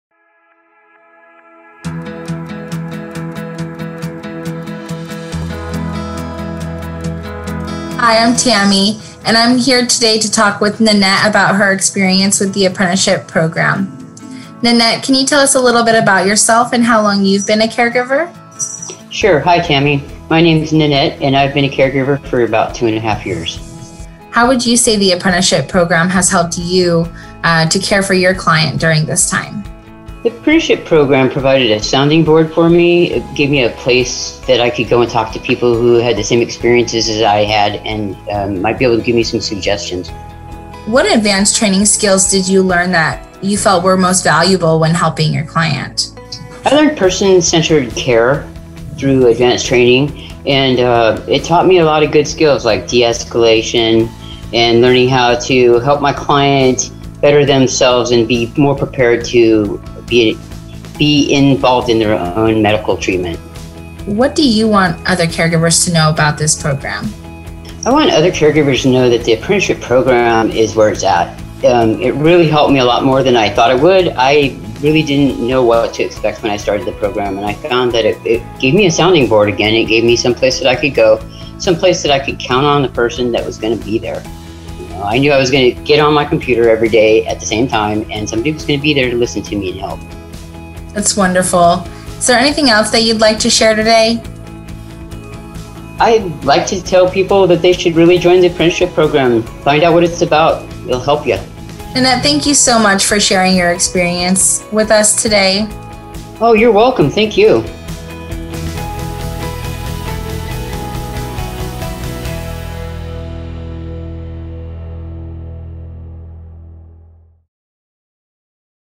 Apprenticeship Week 2020 - Nanette's Story
Nanette W. talks to Tammy J. about her experience with the Apprenticeship Program.